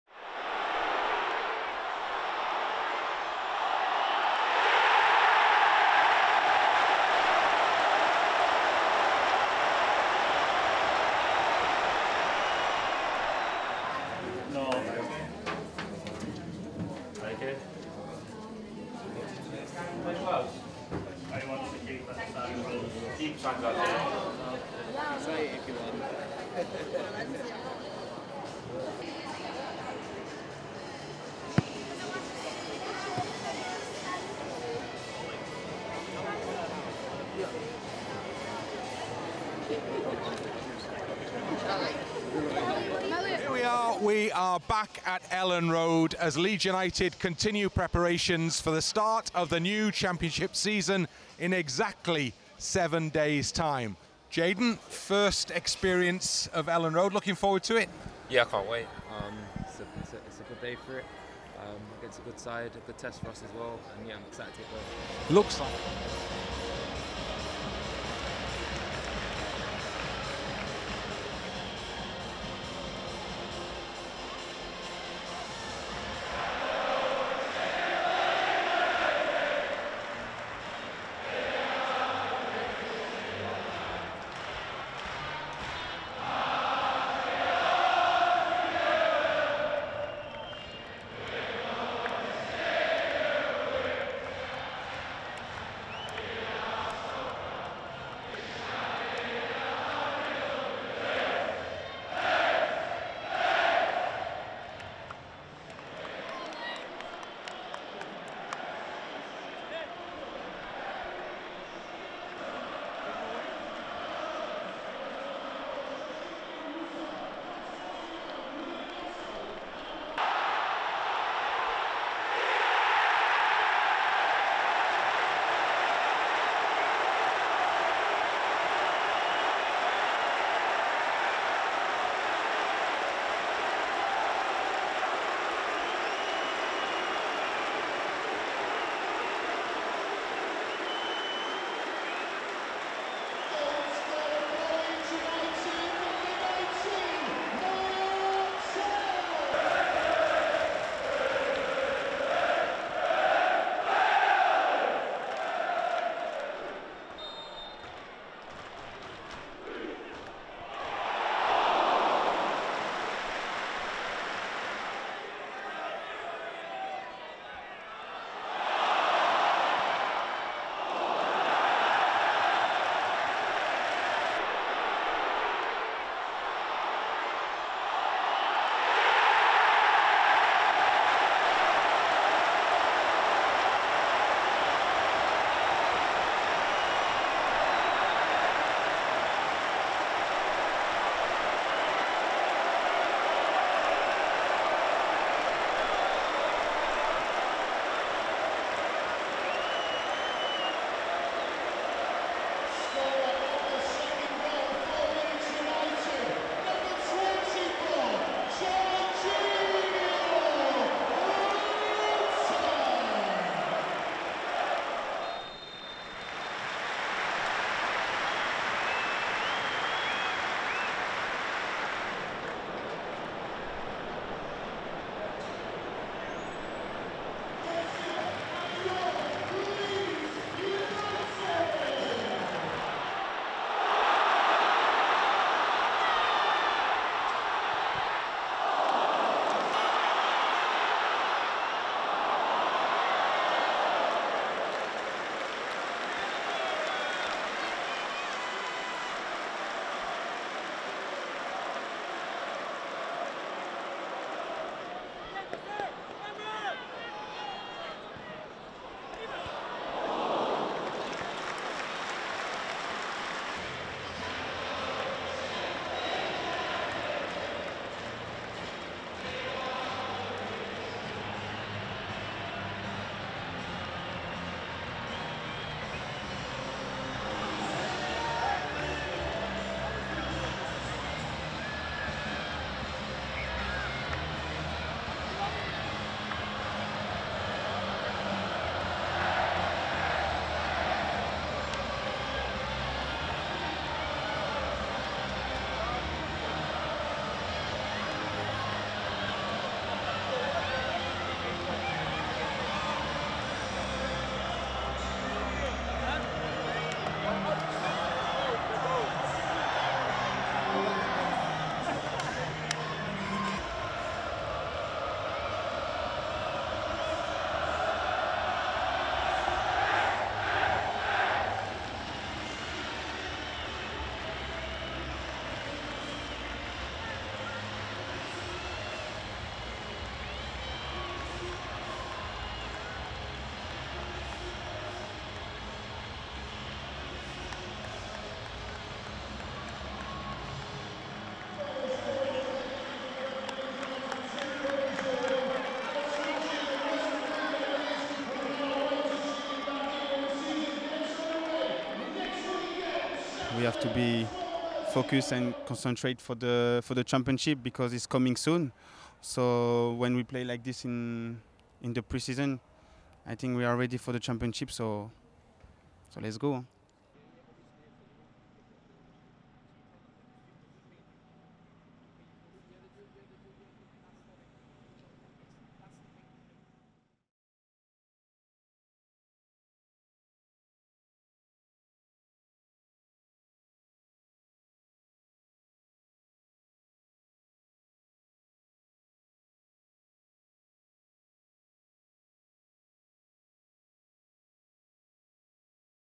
Elland Road win over Valencia! | Uncut
Exclusive and behind-the-scenes footage of our final pre-season game as we hosted Valencia .

Presented by  @HisenseInternational